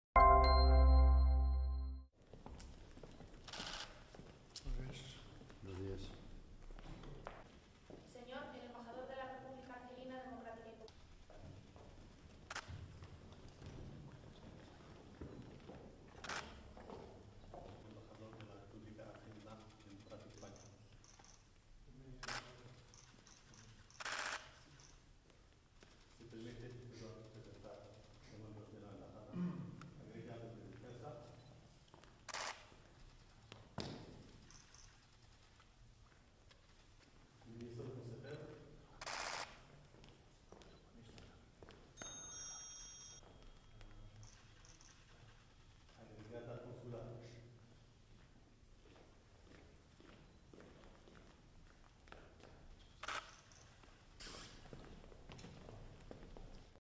El nuevo embajador argelino entrega credenciales al rey de España tras crisis diplomática
(Imágenes: Jesús Bartolomé) El rey Felipe VI ha recibido hoy en el Palacio Real de Madrid la cartas credenciales del nuevo embajador de Argelia, Abdelfetah Daghmoum. 00,56"

INCLUYE RECURSOS DE LA ENTREGA DE LAS CARTAS CREDENCIALES.